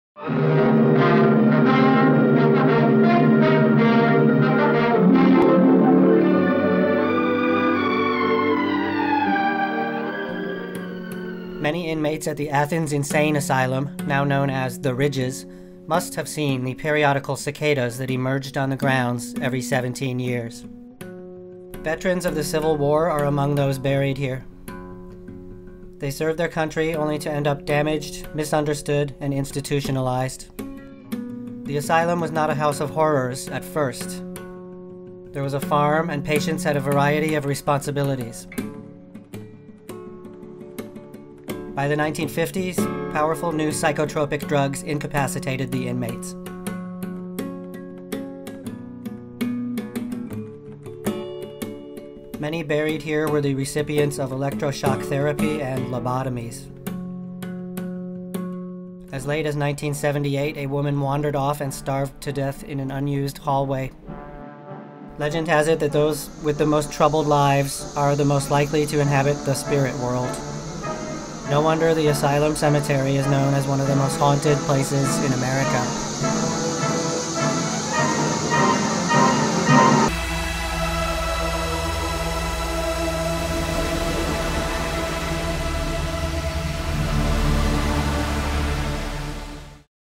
Haunted Insane Asylum Graveyard
Now known as The Ridges, the Athens Insane Asylum has two graveyards. Many of the inmate graves are marked with only a three digit number, "potter's field" style.

"The Athens Lunatic Asylum began operation on January 9, 1874. Within two years of its opening, The hospital was renamed The Athens Hospital for the Insane. Later, the hospital would be called the Athens Asylum for the Insane, the Athens State Hospital, the Southeastern Ohio Mental Health Center, the Athens Mental Health Center, the Athens Mental Health and Mental Retardation Center, the Athens Mental Health and Developmental Center, and then (again) the Athens Mental Health Center."
-Wikipedia